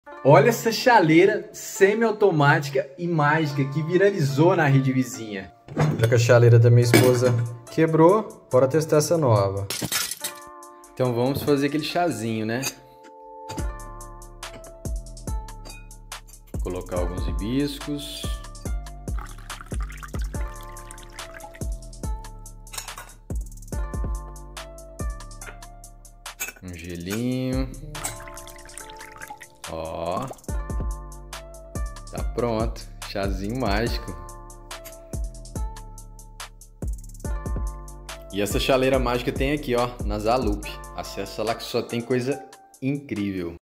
Chaleira Corça Celestial
Chaleira de Vidro com Infusor Corça Celestial, perfeita para momentos de introspecção e deleite, esta chaleira une a estética mística da corça com a tranquilidade do azul profundo, prometendo transformar suas cerimônias de chá em experiências enriquecedoras e serenas.